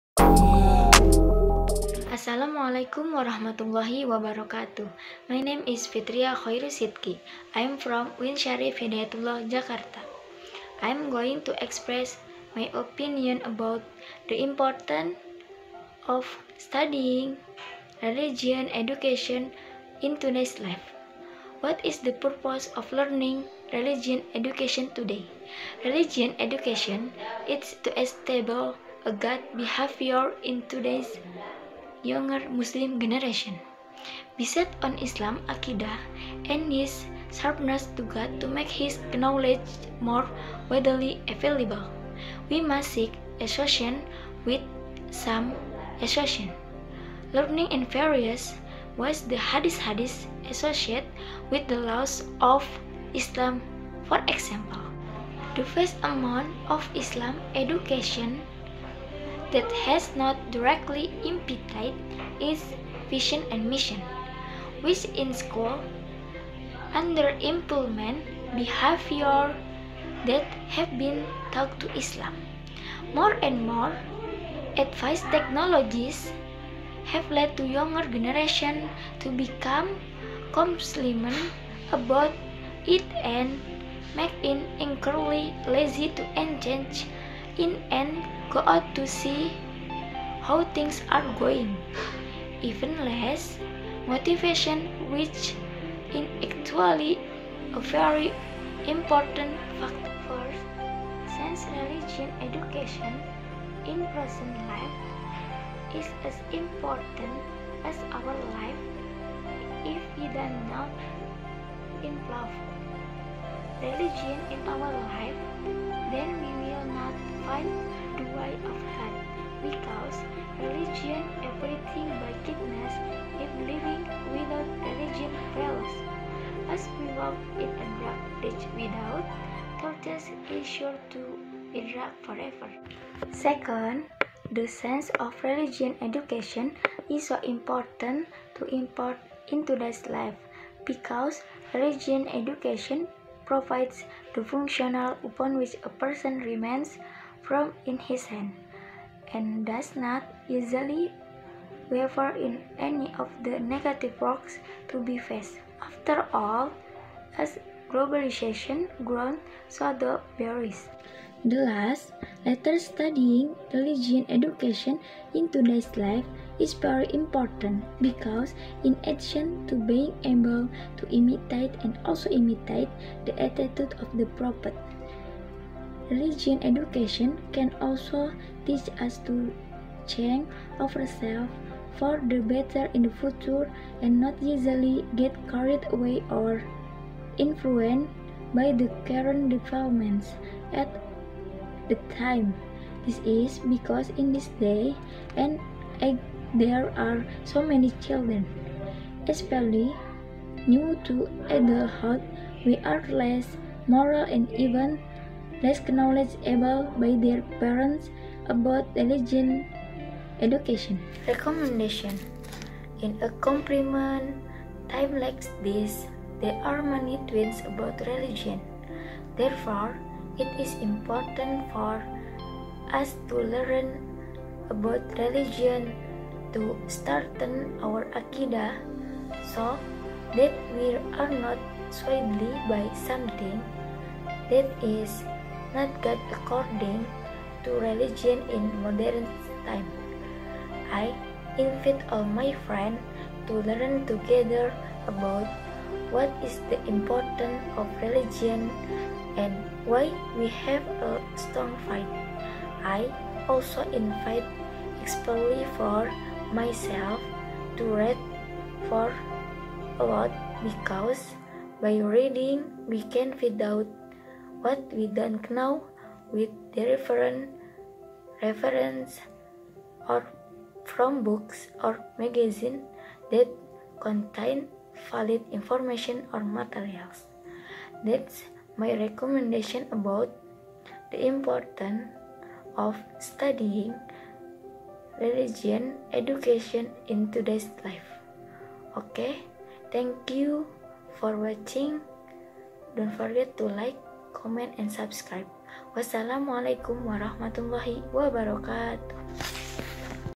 " The importance of studying religious education in today's life "
NAMA : FITRIA KHOIRU SIDQI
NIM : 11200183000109
KELAS : 1C
PRODI : PGMI

Thesis

What is the purpose of learning religious education today ? religious education aims to establish a good behavior in today's younger Muslim generations, based on islamic aqidah and its sharpness to god to make his knowledge more widely available, we must seek association with wholesome associates, learning in various ways the hadits-hadits associated with the laws of Islam, for example. Islam aims to instill a very important and useful public soul in the future The barriers that occur in life vary and may increase over time, such as a very weak consciousness.

 The vast amount of islamic education that has not directly impacted its vision and mission, which in school has underimplemented the behaviors that have been taught to Islam. More and more advanced technologies have led the younger generation to become complacent about it and make it increasingly lazy to engage in and go out to see how things are going, even less motivation which is actually a very important factor.

 He said the importance of islamic religious education in the younger generation is to realize the ideals of islamic society that are in accordance with the commands of god SWT, and to instill achlakul karimah as a guide to the path that god has prepared.

Arguments

First, since religious education in present life is as important as our lives if we do not involve religion in our lives then we will not find the way of good because religion teaches everything by kindness if living without religious values as we walk in a dark place without torches is sure to be dark forever. And can form a purna that can eventually draw closer to god and form the perfect soul to obtain the happiness of the world and the afterlife.

Second, the science of religious education is so important to impart in today's life. Because religious education provides the foundation upon which a person remains firm in his stand and does not easily waver in any of the negative works to be faced. After all, as globalization grows, so do barriers.

The last, latter studying religious education in today's life is very important because in addition to being able to imitate and also imitate the attitude of the Prophet, religious education can also teach us to change ourselves for the better in the future and not easily get carried away or influenced by the current developments at the time. This is because in this day and age there are so many children, especially new to adulthood, who are less moral and even less knowledgeable by their parents about religious education.

Recommendation

In a complicated time like this there are many tweets about religion, therefore it is important for us to learn about religion to strengthen our aqidah so that we are not swayed by something that is not good according to religion in modern times, I invite all my friends to learn together about what it is the importance of religion and why we have a strong faith I also invite especially for myself to read a lot because by reading we can find out what we don't know with definite references, or from books or magazine that contain valid information or materials.

Video ini dibuat untuk memenuhi tugas mata kuliah Bahasa Inggris - PGMI yang diampu Ibu Viviana Lisma Lestari, M. Pd.